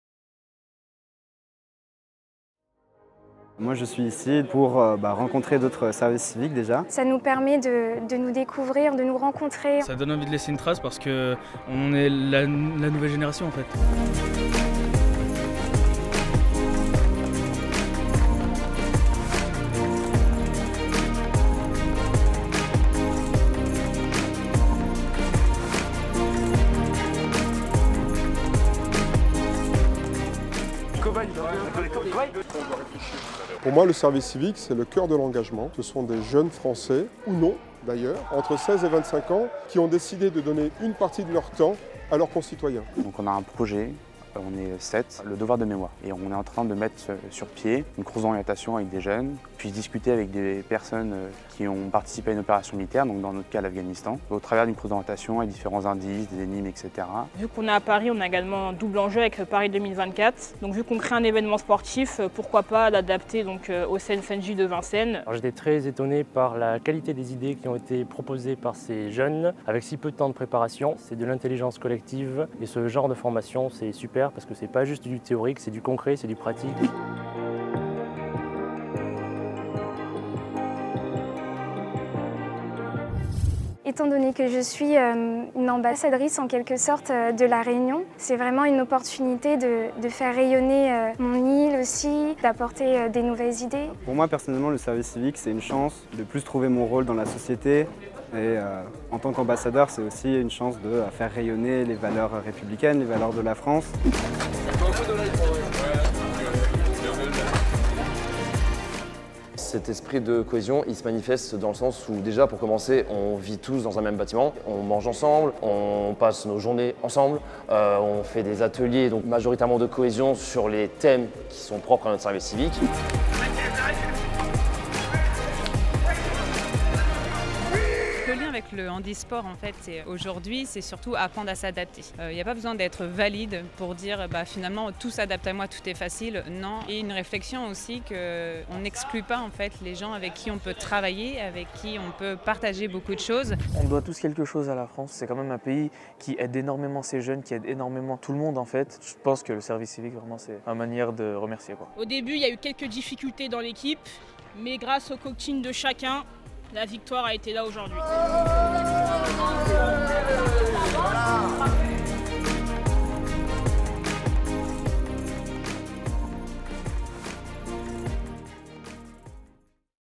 Premier stage pour la promotion 2021-22 des volontaires du service civique du ministère des Armées
Fin 2021, une nouvelle promotion de volontaires du service civique (VSC) du ministère des Armées s'est réunie aux Ecoles Militaires de Bourges pour un premier stage marquant le début de leur engagement au service du lien armées-jeunesse. 43 volontaires de métropole et d’outre-mer ont découvert ensemble le monde militaire au travers de jeux interactifs et de rencontres avec les partenaires de cette mission. Au programme : Plan ambition armées-jeunesse, dispositif Aux sports jeunes citoyens ! et dispositifs jeunesse du ministère des Armées. Une semaine mémorable de cohésion, de sport et de formation sur fond d’échanges sur le lien armées-jeunesse.